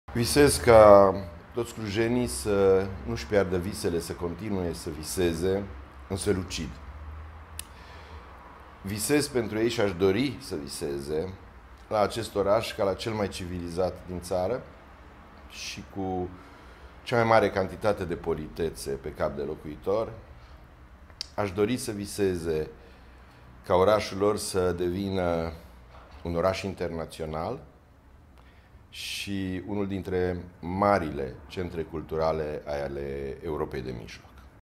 Mihai Măniuțiu - manager Teatrul Național Cluj-Napoca - 100 de vise pentru Cluj
Managerul Teatrului Național Cluj-Napoca, Mihai Măniuțiu, își dorește ca toți clujenii să viseze la acest oraș ca la cel mai civilizat din țară și cu cea mai mare cantitate de politețe pe cap de locuitor. Clujul ar trebui să devină un oraș internațional și unul dintre marile centre culturale ale Europei de Mijloc.